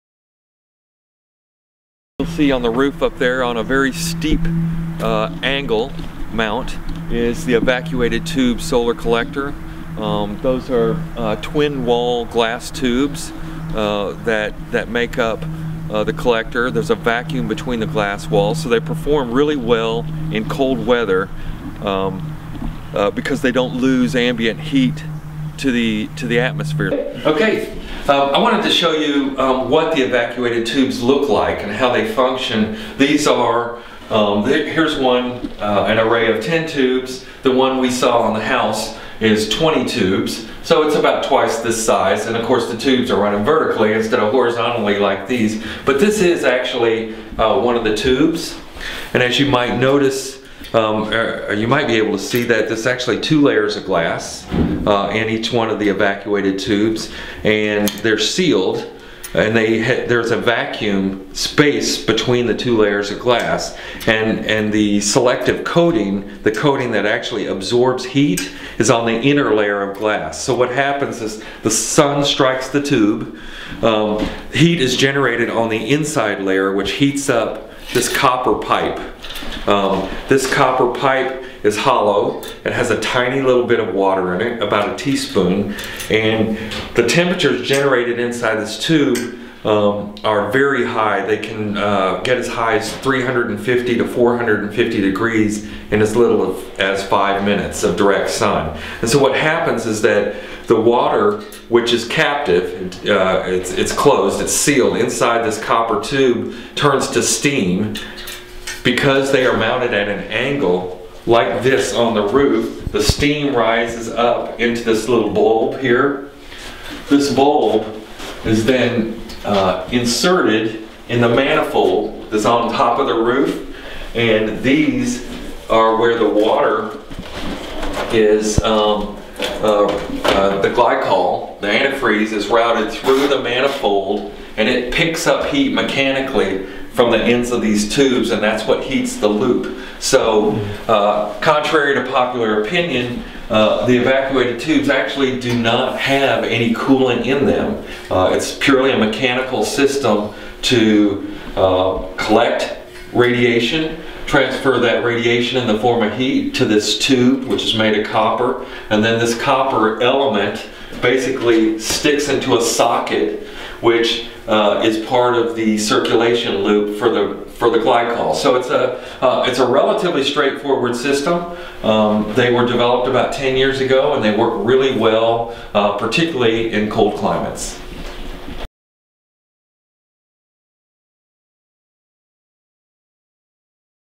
solar residential - evacuated tubes
Learn about how evacuated tubes are used to produce hot water using only sunlight. These tubes work well in colder climates.